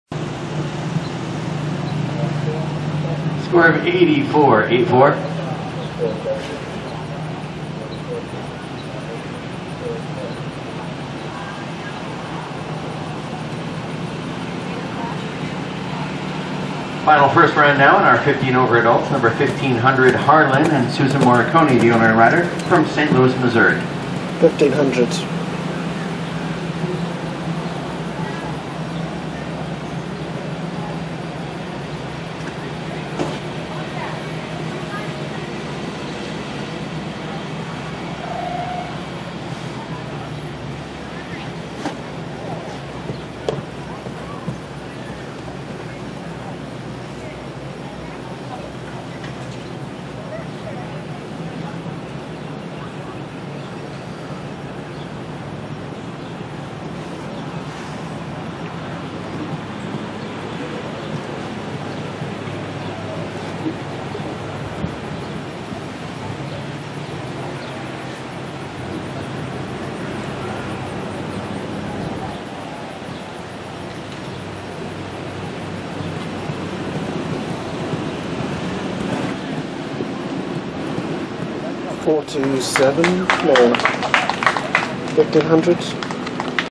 Video of HARLAN ridden by SUSAN MORICONI from ShowNet!
HARLAN ridden by SUSAN MORICONI at Kentucky Summer Horse Show 2015.  This round is from class 170, ADULT AMATEUR HUNTER 50 & O and finished 3 of 10.